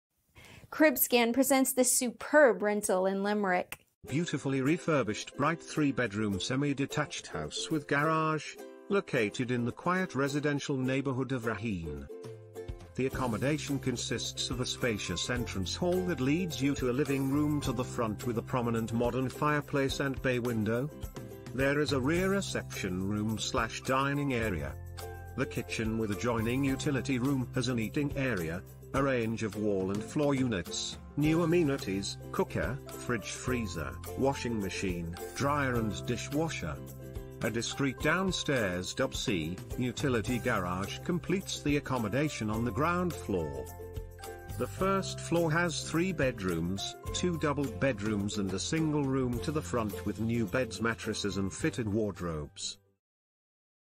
House to rent in Limerick, Raheen, €1,200 / monthly
3
1
Central heating

REF49857
Beautifully refurbished bright 3 bedroom Semi-Detached...